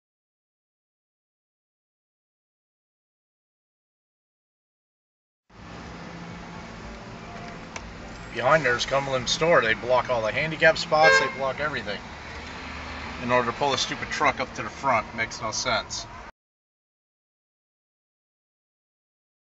Cumberland Farms & Deliveries Chasing Business Away
I have sympathy for the delivery drivers: they have to wait for the spaces to clear, while blocking traffic on the main road in order to do their job by inching into these spots